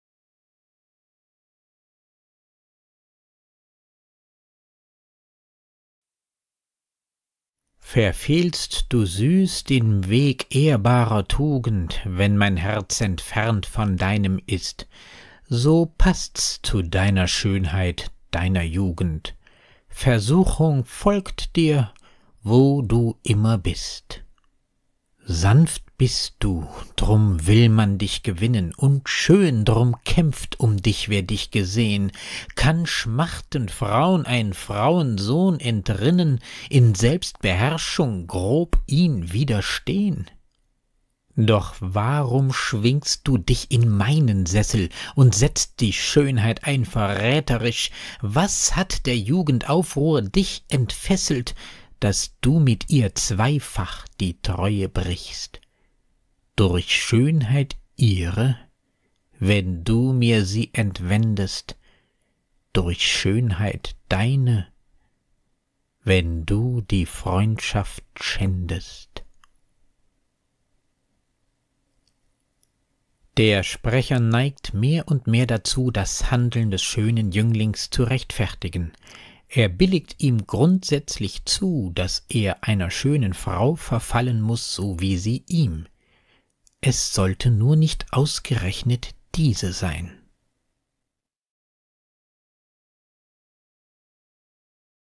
Shakespeare Sonett 41: Those pretty wrongs (deutsch) - Eigene Nachdichtung
Der Sprecher neigt mehr und mehr dazu, das Handeln des schönen Jünglings zu rechtfertigen. Er billigt ihm grundsätzlich zu, dass er einer schönen Frau verfallen muss, so wie sie ihm. Es sollte nur nicht ausgerechnet diese sein.